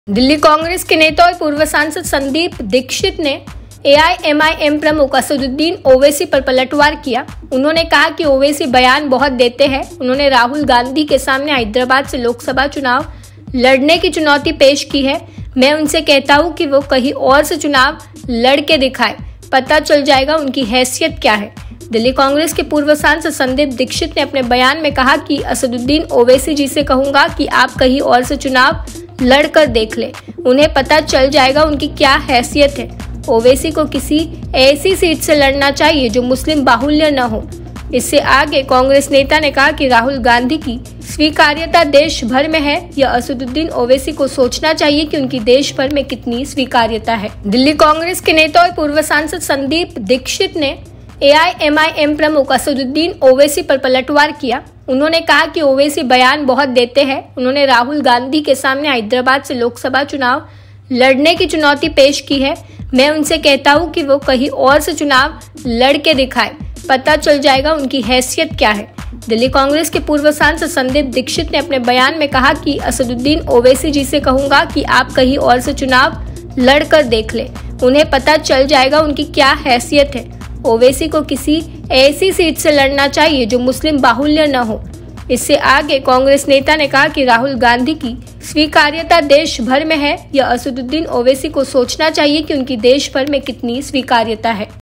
Sandeep Dixit का ओवैसी पर पलटवार, AIMIM चीफ गैर मुस्लिम सीट से चुनाव लड़कर दिखाएं
दिल्ली कांग्रेस के नेता और पूर्व सांसद संदीप दीक्षित (Sandeep Dixit) ने एआईएमआईएम प्रमुख असदुद्दीन ओवैसी (AIMIM Chief Asaduddin Owaisi) पर पलटवार किया है. उन्होंने कहा कि ओवैसी बयान बहुत देते हैं. उन्होंने राहुल गांधी (Rahul Gandhi) के सामने हैदराबाद से लोकसभा चुनाव (Lok Sabha Election 2024) लड़ने की चुनौती पेश की है. मैं, उनसे कहता हूं, वो कहीं और से चुनाव लड़के दिखाएं, पता चल जाएगा उनकी हैसियत क्या है?दिल्ली कांग्रेस के पूर्व सांसद संदीप दीक्षित ने अपने बयान में कहा है कि असदुद्दीन ओवैसी जी से कहूंगा की आप कहीं और से चुनाव लड़कर देख लें, उन्हें पता चल जाएगा की उनकी क्या हैसियत है. ओवैसी को किसी ऐसी सीट से लड़ना चाहिए जो मुस्लिम बाहुल्य न हो. इससे आगे कांग्रेस नेता ने कहा कि राहुल गांधी की स्वीकार्यता देशभर में है. यह असदुद्दीन ओवैसी को सोचना चाहिए की उनकी देश भर में कितनी स्वीकार्यता है.